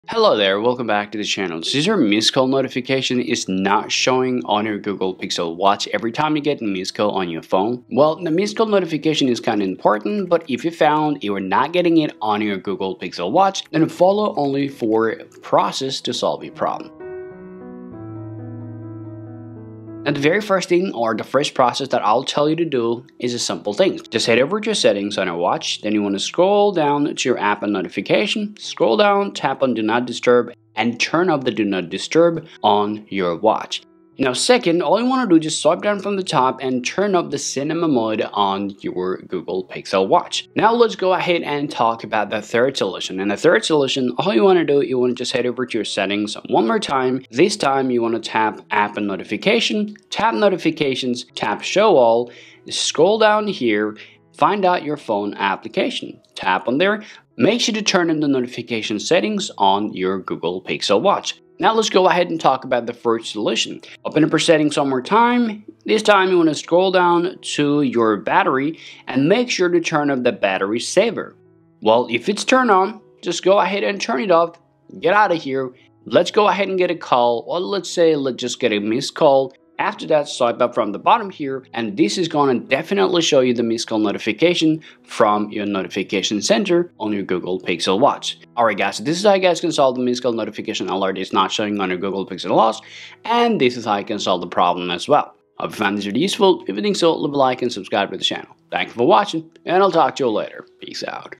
Pixel Watch: How To Fix Missed Call Notification Not Showing! [Not Coming]
Missed Call Notifications not coming on your Google Pixel Watch Notification Center, so you are missing out on important call alerts unless you check your phone? The 4 easy solutions to fix Missed Call Notifications not showing issues on Google Pixel Watch.

0:00 Missed call notification missing problem
0:20 Solution 1: Turn Off the Do Not Disturb mode
0:34 Solution 2: Turn Off Cinema Mode
0:43 Solution 3: Turn On the Phone Notification
1:04 Solution 4: Turn Off Battery Saver